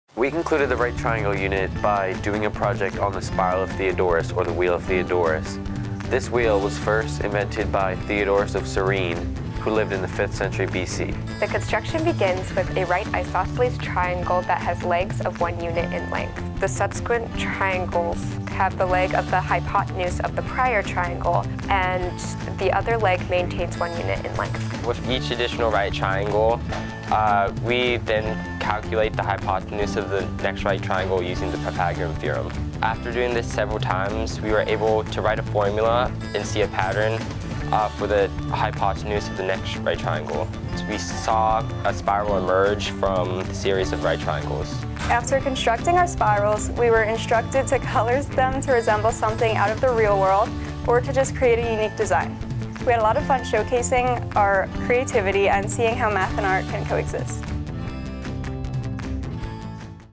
Wheel of Theodorus Math Project
Eighth-grade algebra students explain how they make the Wheel of Theodorus, a spiral composed of right triangles placed edge-to-edge.